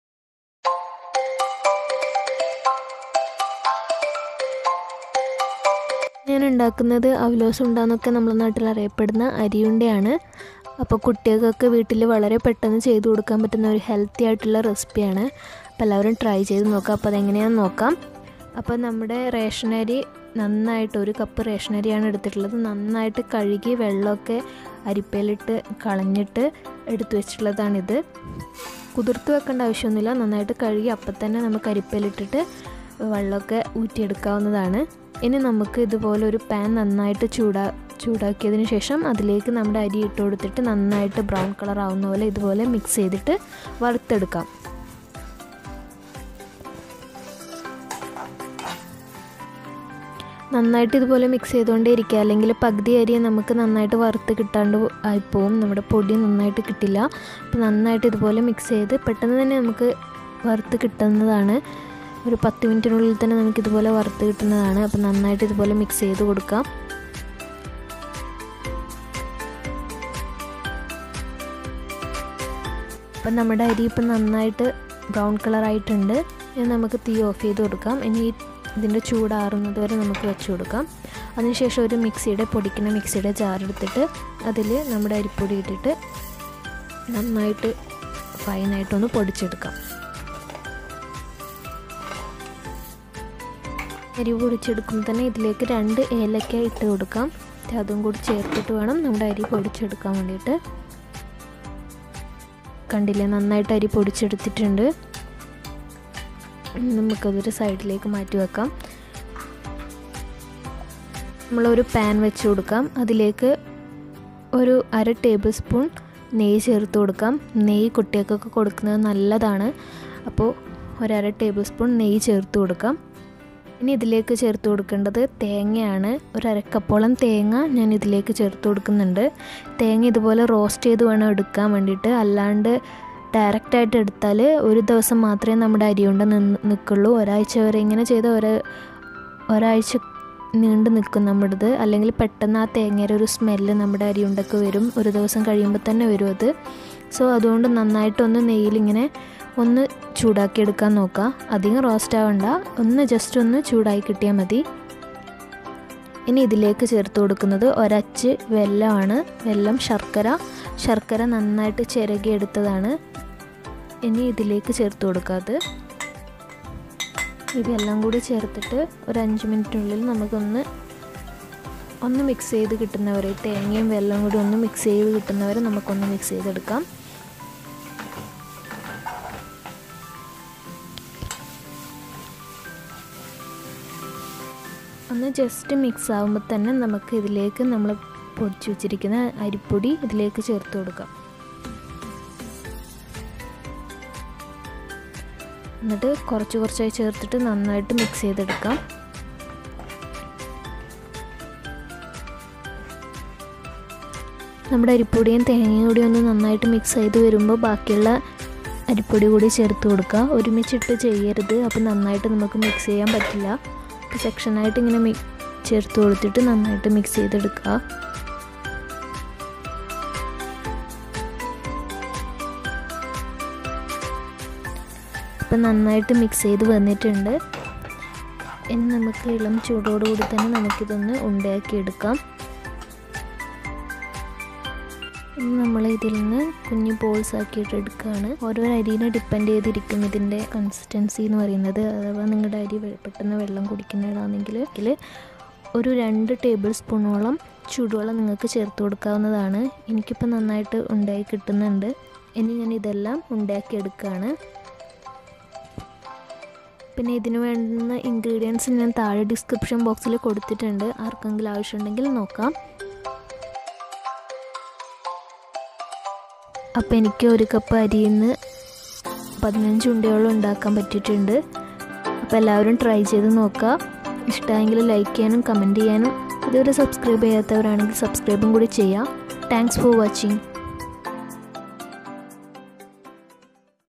Traditional Kerala Snack-Ari Unda|| Avalose Unda Recipe ||അവലോസുണ്ട
Ingredients required
Rice-1cup
Coconut scraped-1/2 cup
Cardamom-2
Jaggery-1
Ghee-1/2 tsp